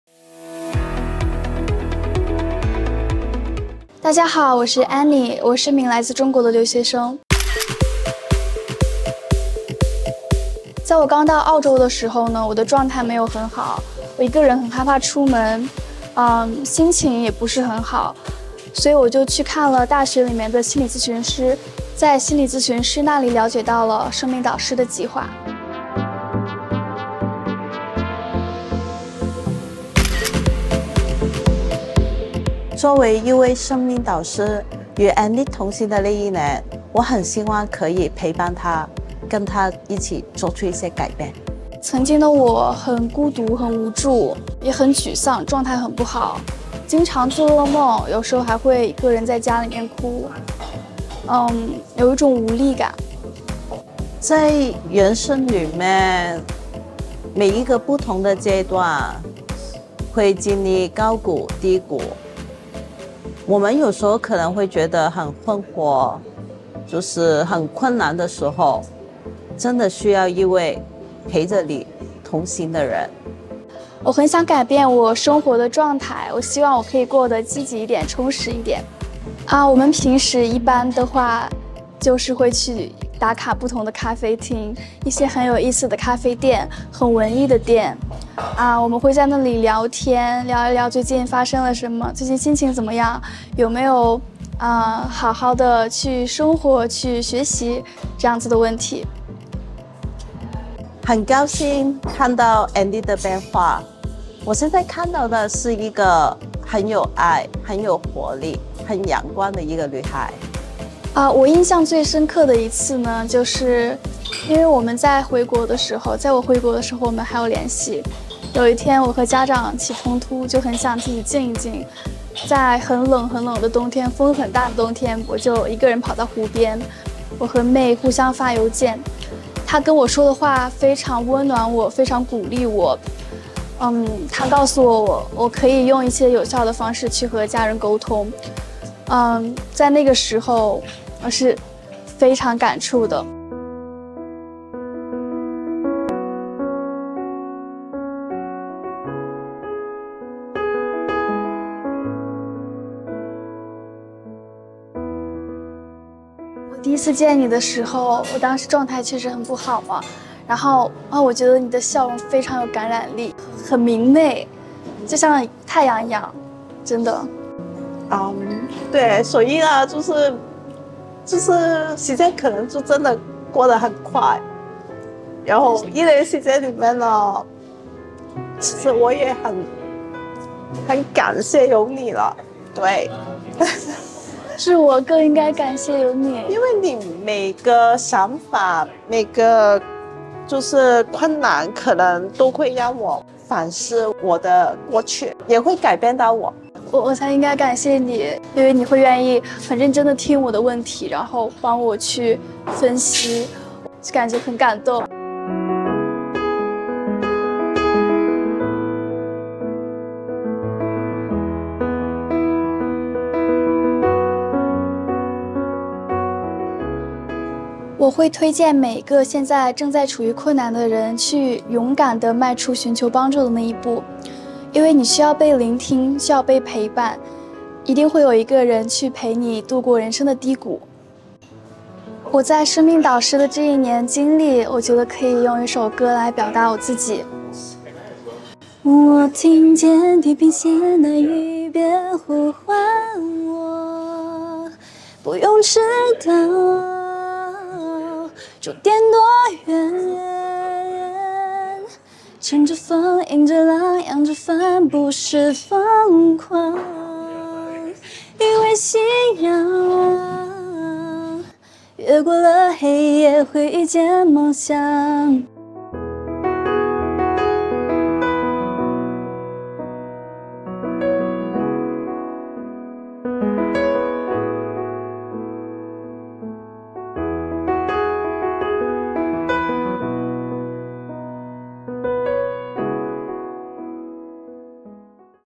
Mei & Anni｜生命導師 (English & Traditional Chinese Subtitle Version)
困境加孤獨令人有沮喪和失敗感，但若有一個可信任的人予以同行， 聆聽受助者的處境、嘗試指導受助者尋找走出困境的路道和協助尋找合適的實質性援助、達成短期或長期的人生目標，極大可能改寫人生。「生命導師：同行成長計劃」是由 COACH （Creating Opportunity and Casting Hope） Community Mentoring「生命導師」 主辦，非盈利組織，成立了二十年。在澳洲和英國超過有130多個地點提供「生命導師」服務，組織是基督教背景，受助者包括英語、普通話和廣東話人仕。他們不單止一對一提供家庭指導、青年指導、兒童指導和賦予能力的社區，更招募有志自願作生命導師的幫助者，篩選和培訓他們成為這個計劃的義務人員，幫助社群、創造機會、燃點希望、建立愛心。

各位讀者如想參與「生命導師：同行成長計劃」，成為生命導師、受助者或伙伴教會，可以聯繫何俊文：